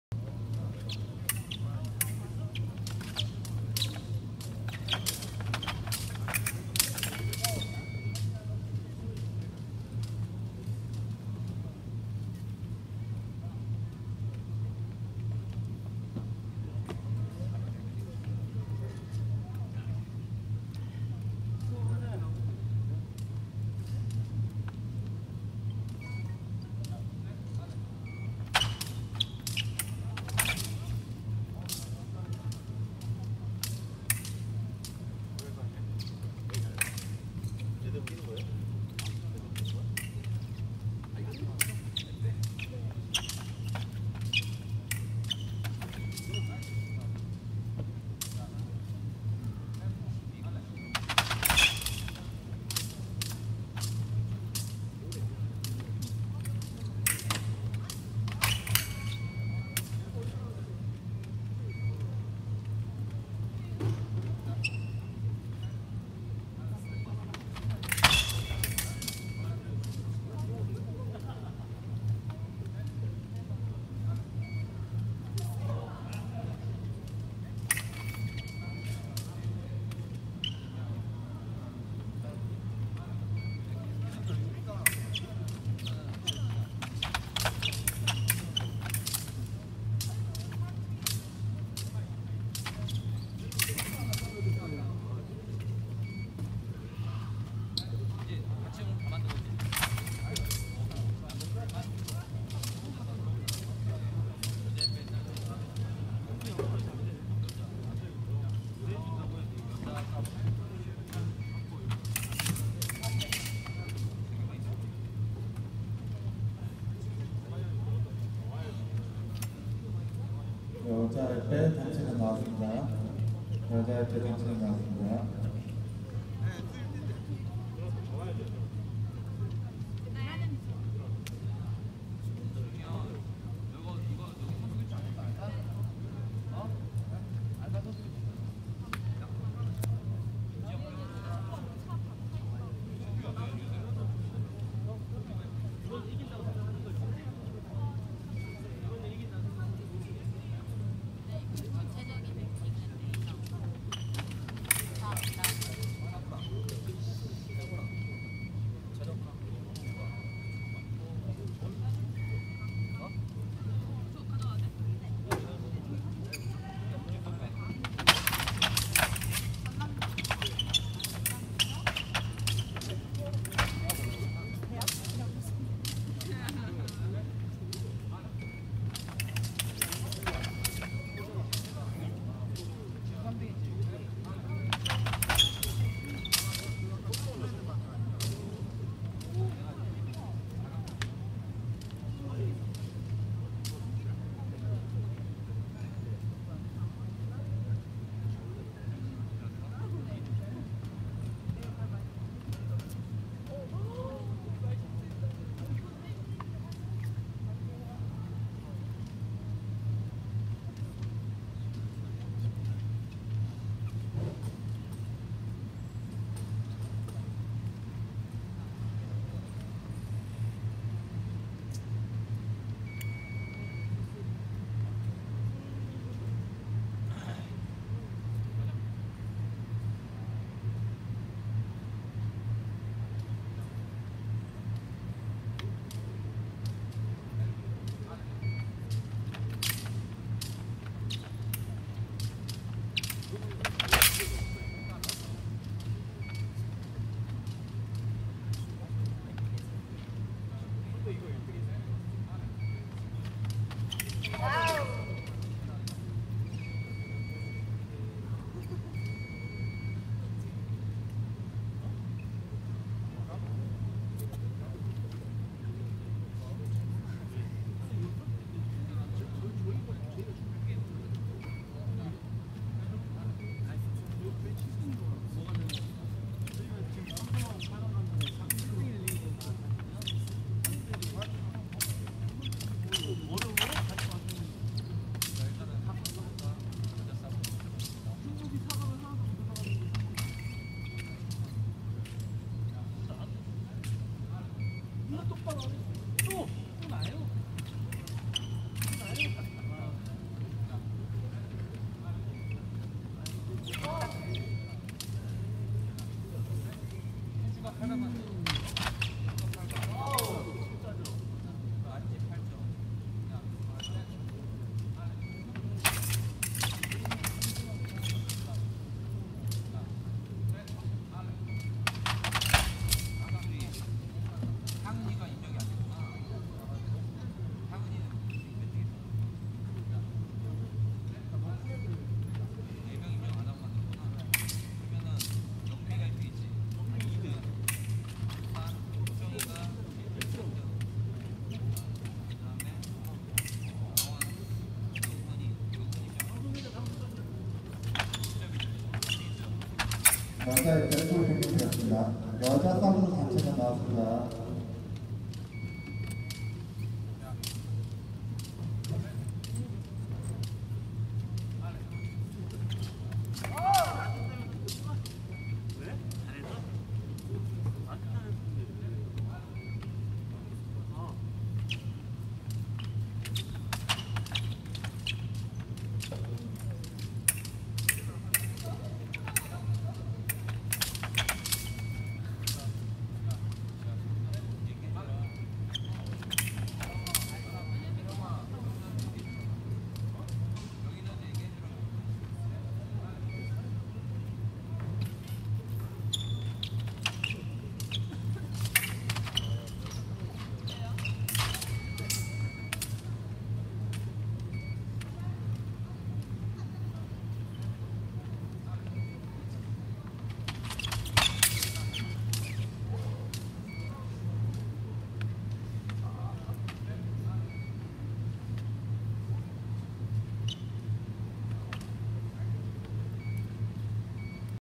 18' Korea national fencing foil tournament final Lee Kwanghyun vs Kim Minkyu 2/3
Korea national fencing foil tournament
Lee Kwanghyun vs Kim Minkyu
2018 펜싱 플러레 국가대표 선발전 김창환배
화성시청 이광현 vs 화성시청 김민규